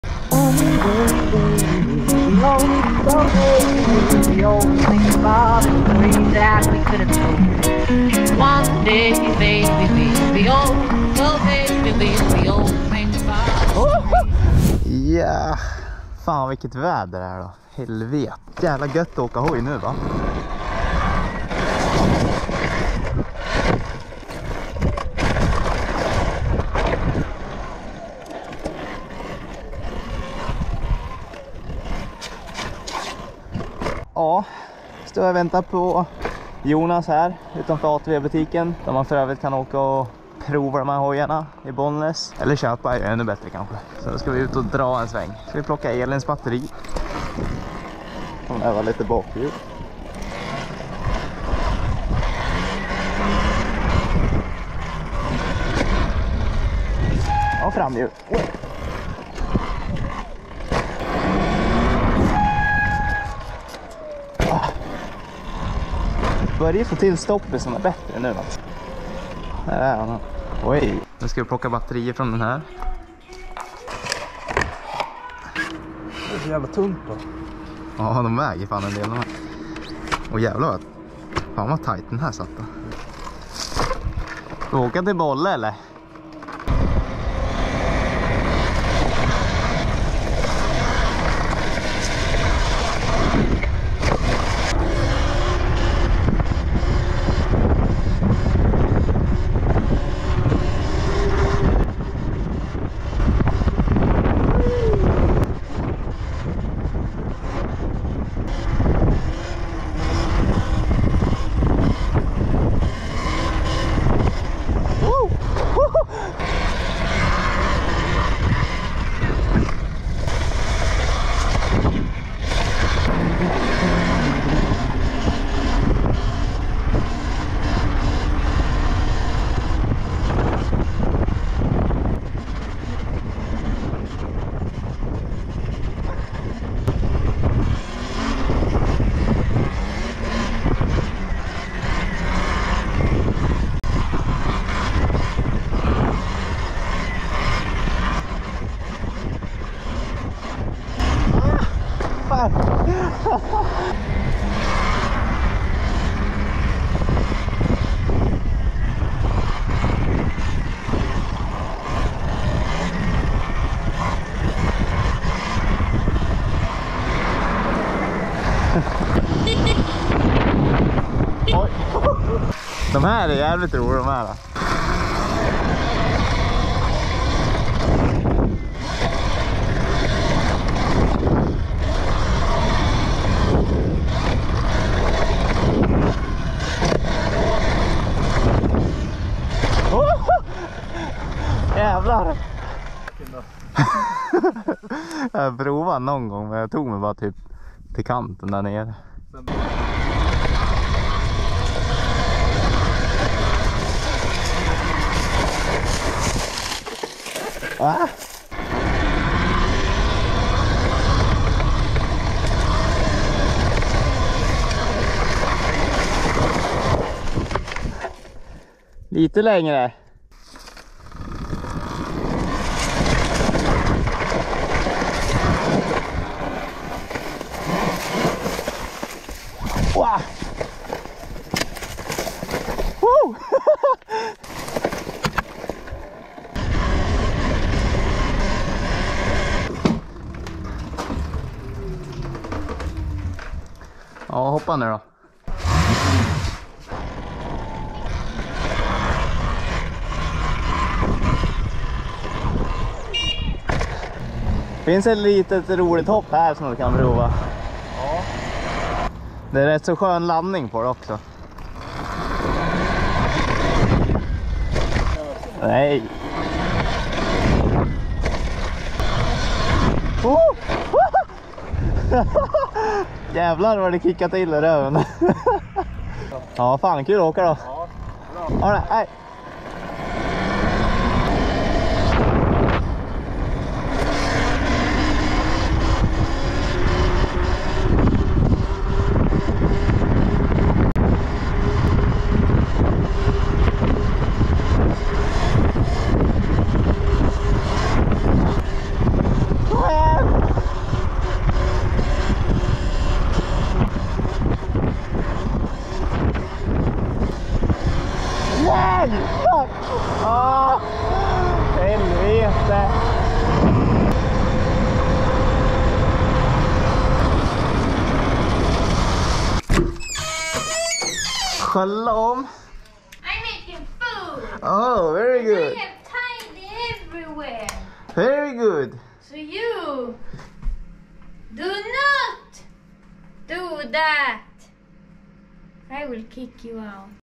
Utforskar Bollnäs på Elhojar - Talaria Vlogg
Äntligen sommar! Ut o glida lite elräcer! Talaria Sting!